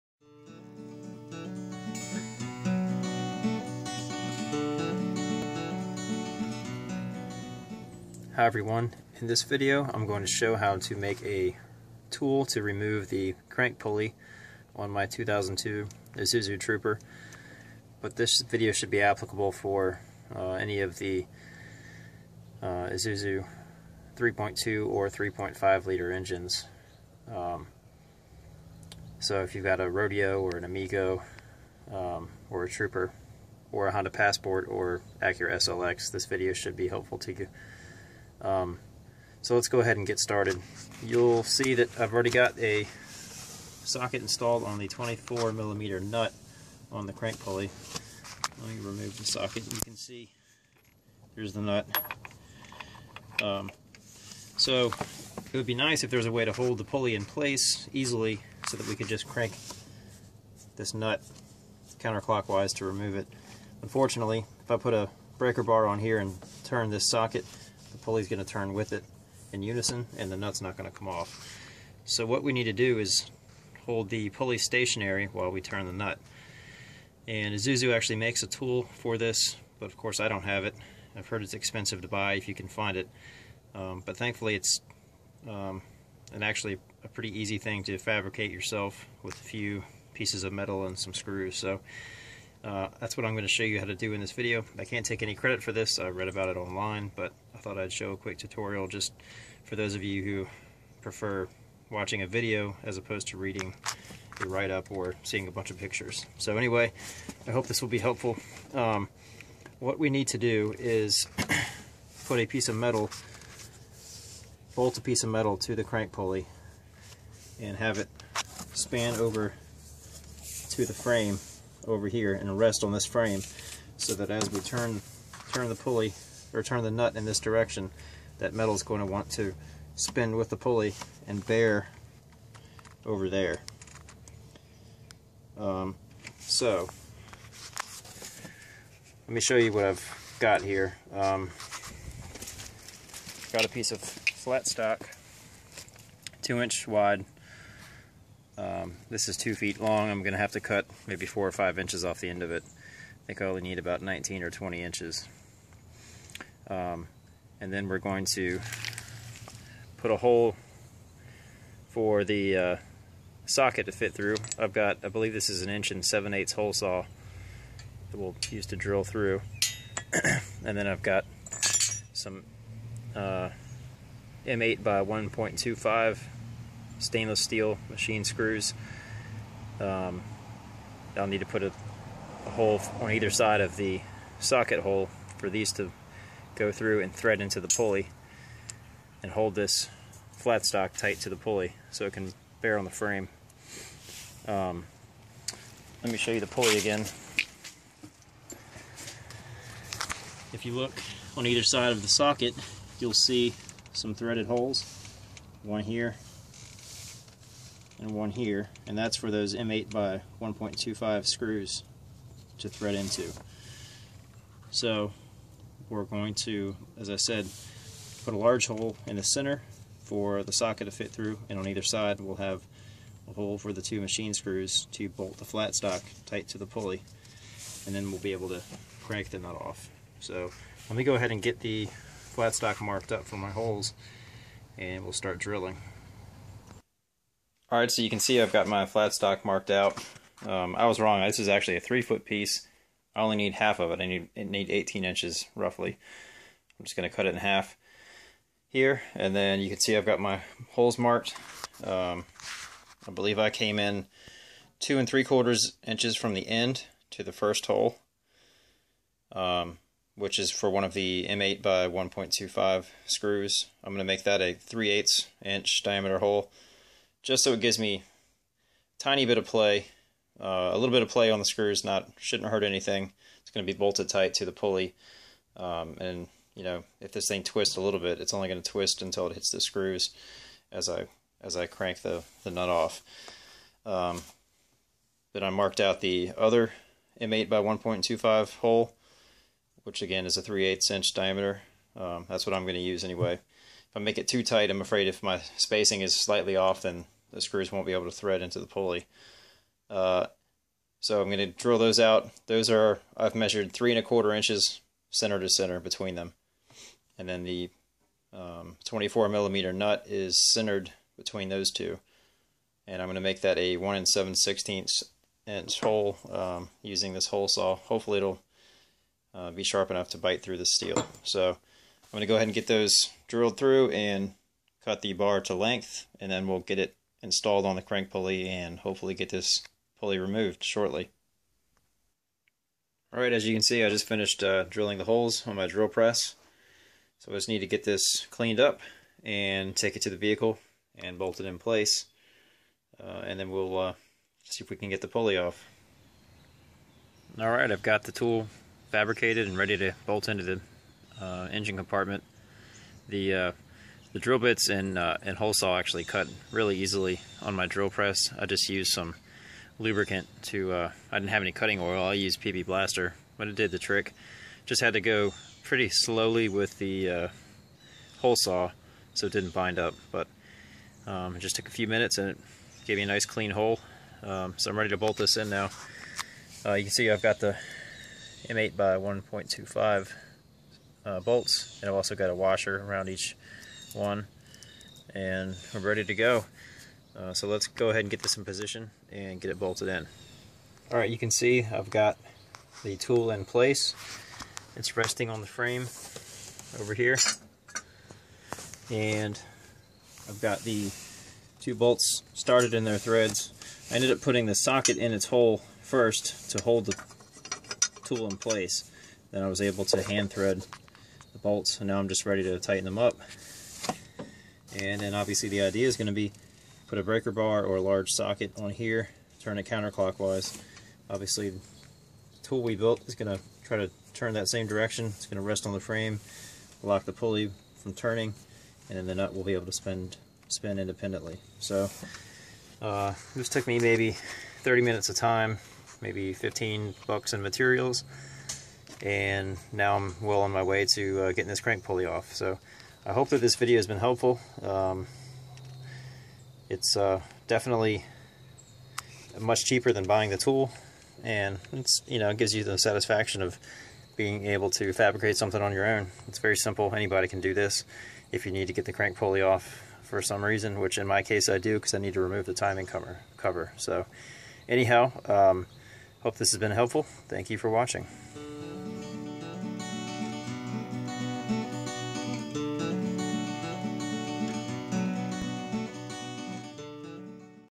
How To Fabricate A Crank Pulley Bolt Removal Tool for Isuzu Engines
In this video, I demonstrate how to fabricate a simple tool to assist in loosening the crankshaft pulley bolt in order to remove the crankshaft pulley from its shaft.  Removing this pulley is a necessary step in replacing the timing belt, as the lower timing cover cannot be removed without first removing the crankshaft pulley.

I am not a professional mechanic, and my videos are not intended to offer professional advice.  Always reference your vehicle's service manual for official information, and please do not attempt any repair that you are not comfortable doing.